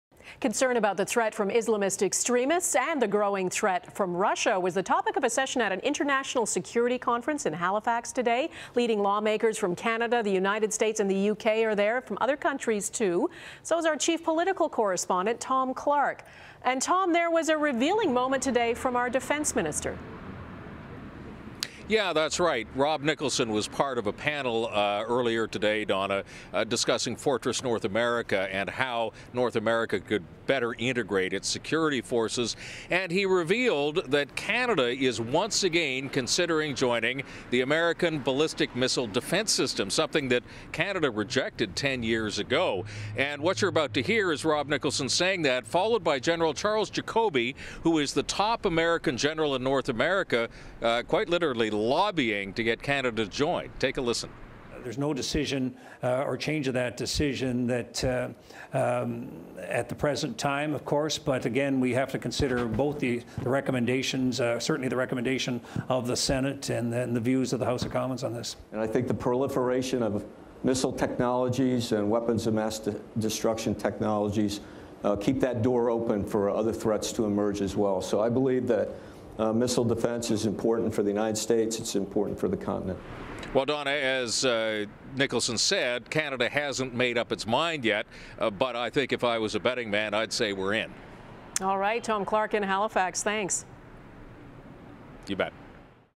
Could Canada join U.S. Ballistic Missile Defence System?
Fri, Nov 21: There was a revealing moment from Defence Minister Rob Nicholson at the Halifax International Security Forum on Friday, where lawmakers from around the world are gathering. Tom Clark joins Dawna Friesen to discuss.